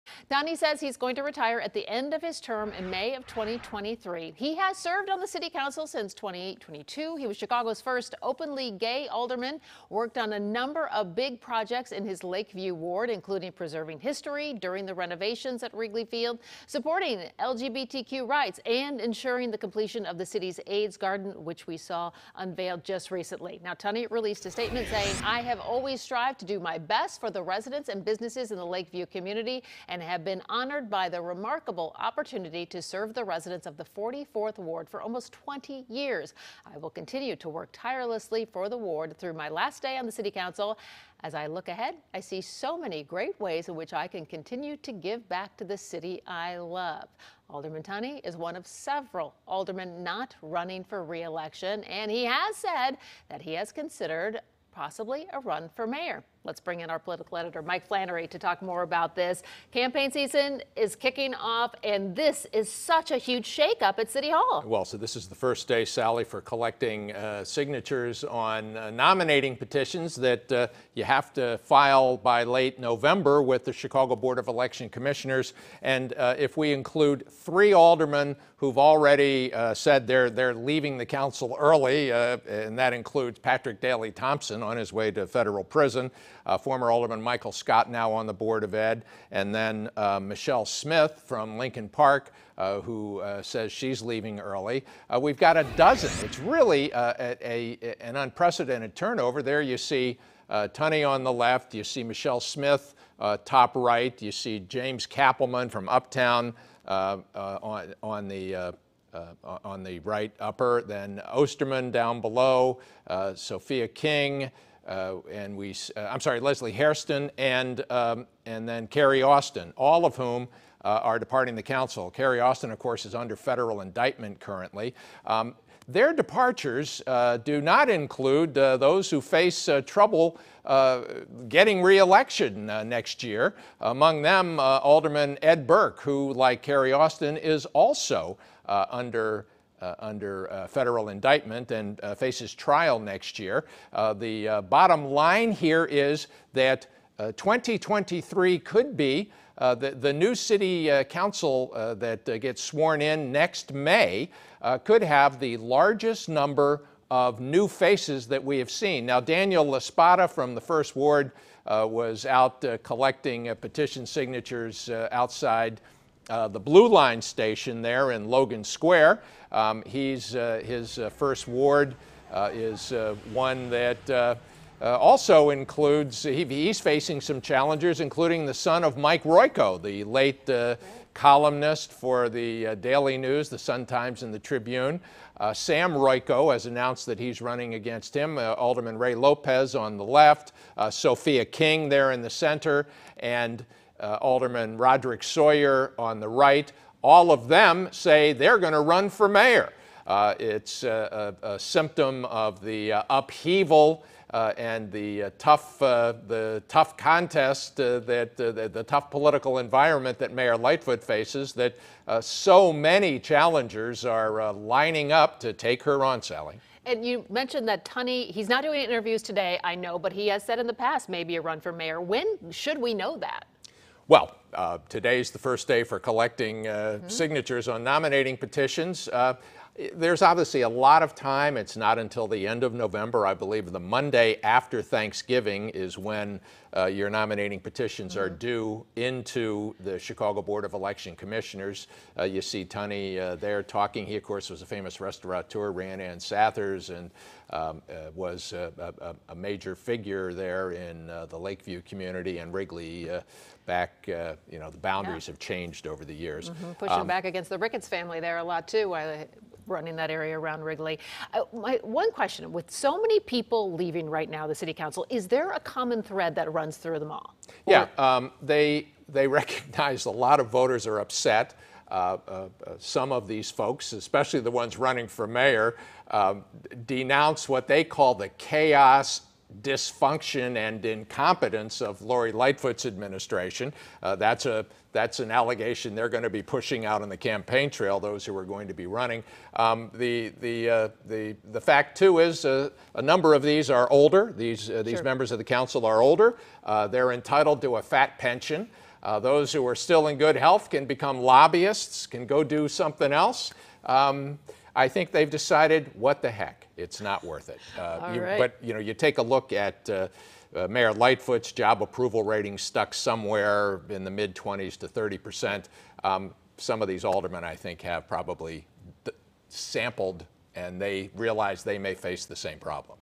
Longtime Ald. Tom Tunney joins City Council exodus
Ald. Tom Tunney (44th), the powerful chairman of the City Council’s Zoning Committee and a key member of Mayor Lori Lightfoot’s leadership team, is retiring.